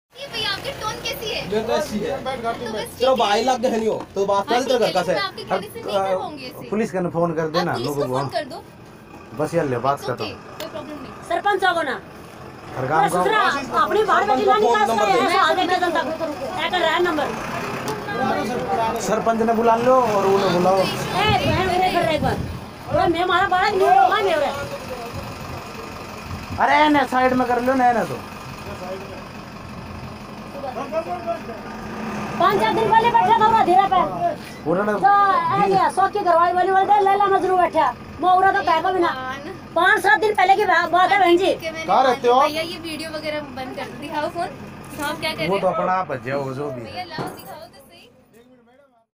Rohtak ki ye ldki jangal me mangal krte pakdi gi.
Rohtak ki ye ldki apne bf ke sath pakdi gi. Fur dekho kya huaa.